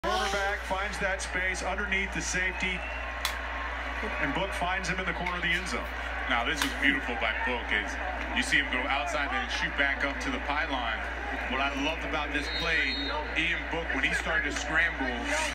Chris Olave First NFL Touchdown ‼️ Vs GreenBay Packers
1st round Rookie Chris Olave First NFL Touchdown ‼️😎